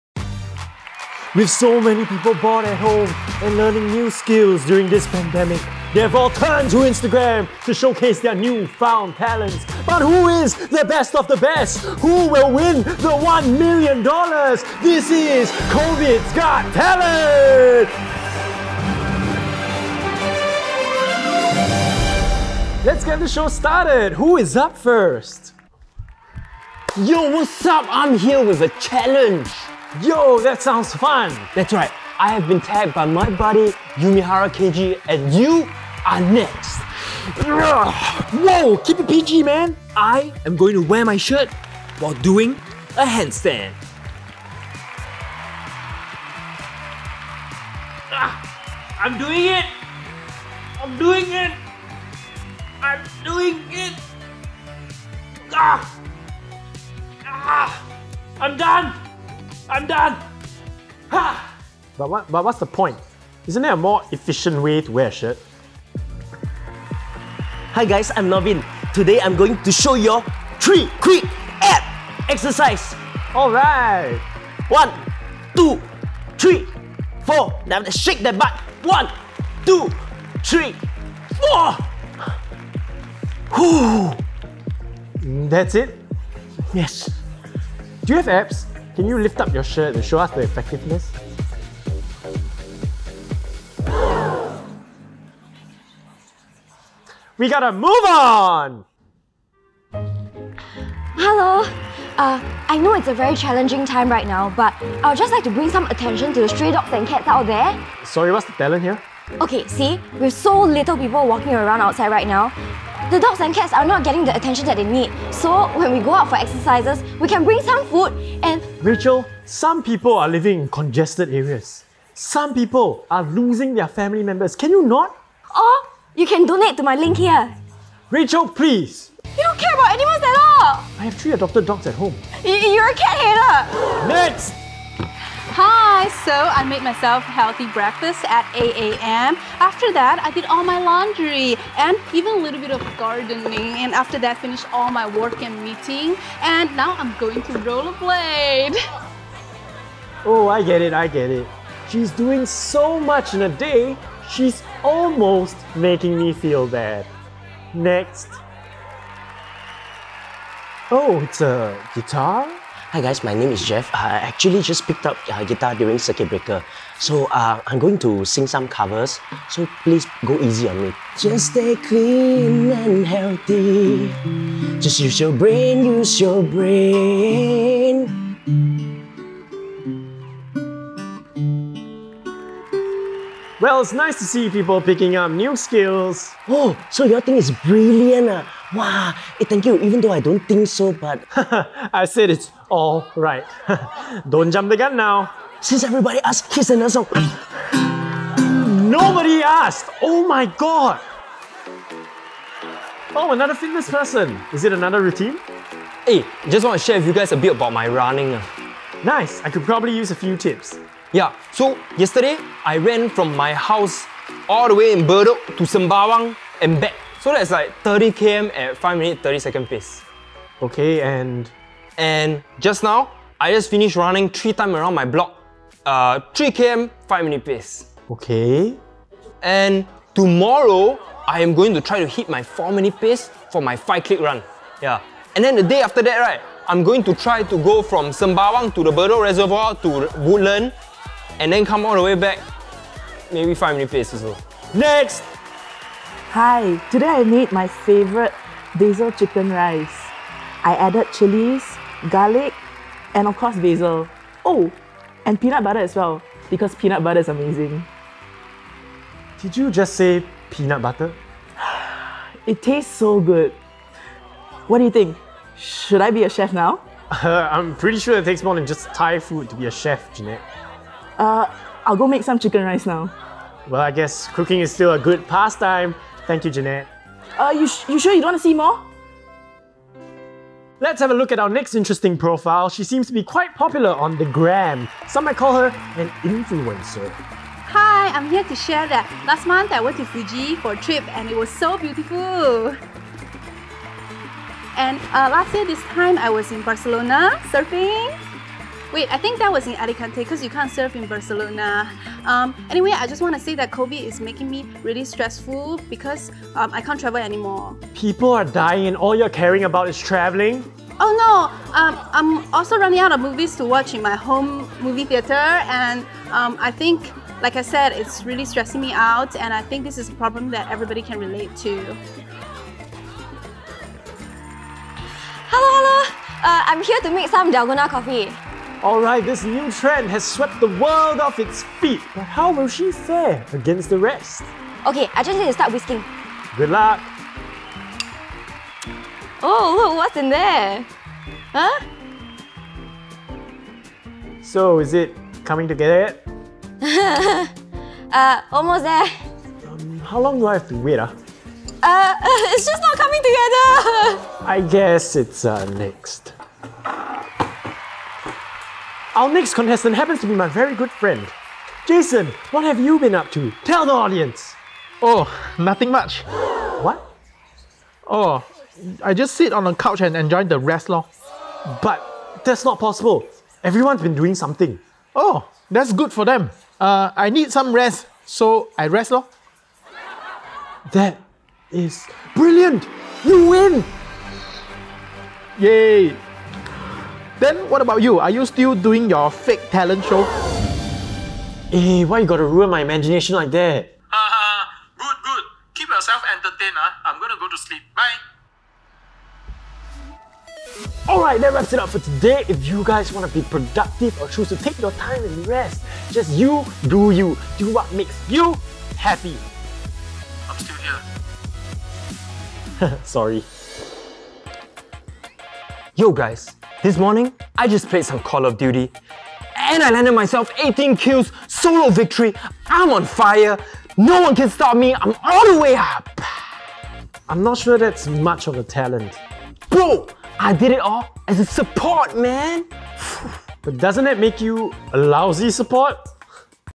Covid's Got Talent
OK we might be taking the piss here but we do appreciate everybody's effort of staying home and entertaining themselves and their loved ones. So as much as this video is a little cynical, we say keep the talents coming guys! Stay safe and love you.

Produced by:
 Jeanette @Jeanettoes

Filmed and edited by:
Justin Siu @JBSIU

Starring:
Lingyi @Blingyi
Thomas @HeyThomasK
Nic @Hargaohunk
Norbin @Stylobin